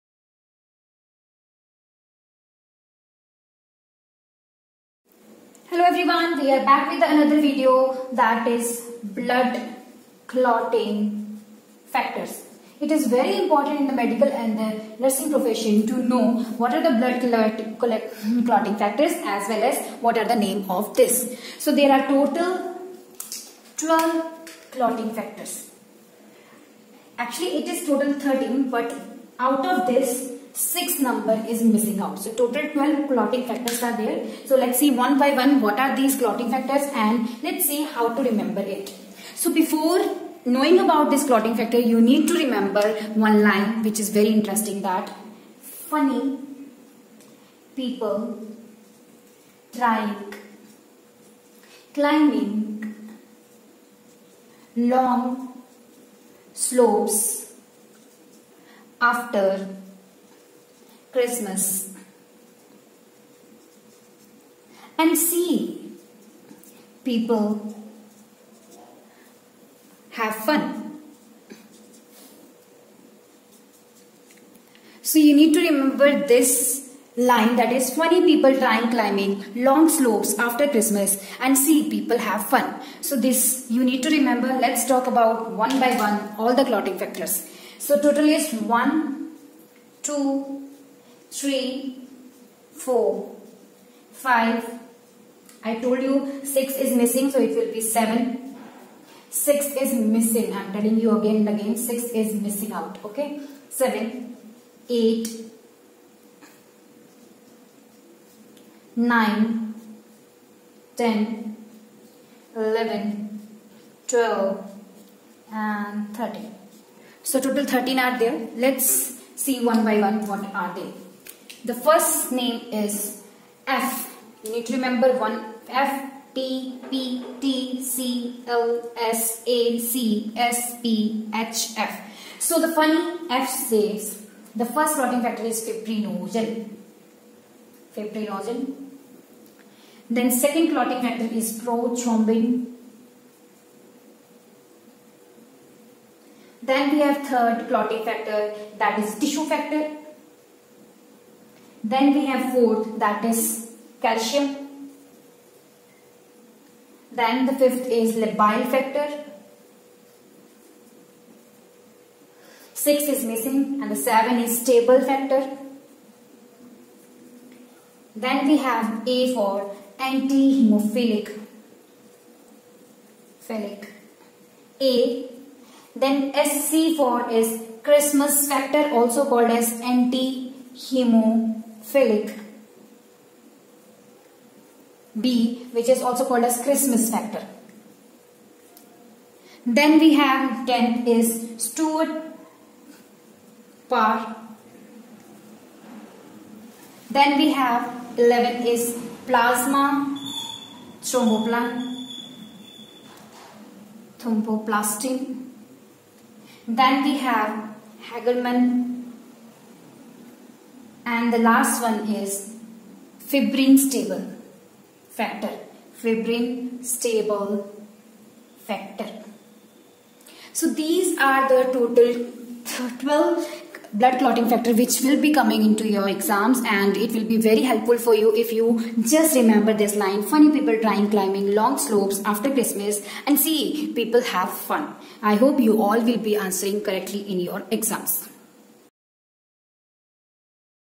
Blood Clotting Factors
Hello everyone , another short video is on how to remember the blood clotting factors